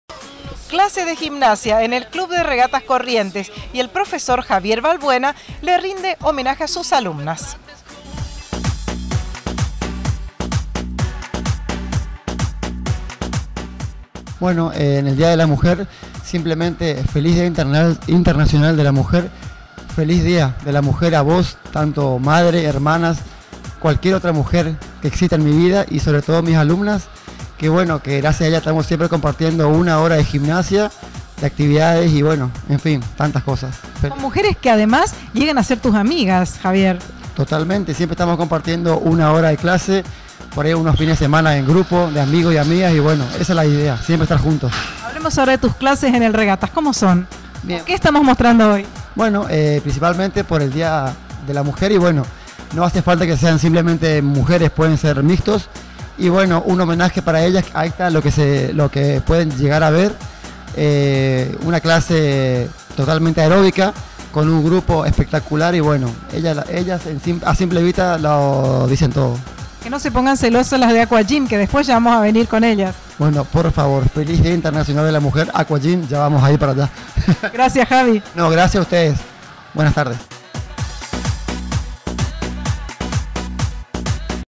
Profesor Javier Valbuena
Saludo del Profe a sus alumnas y amigas por el Día Internacional de la Mujer . . .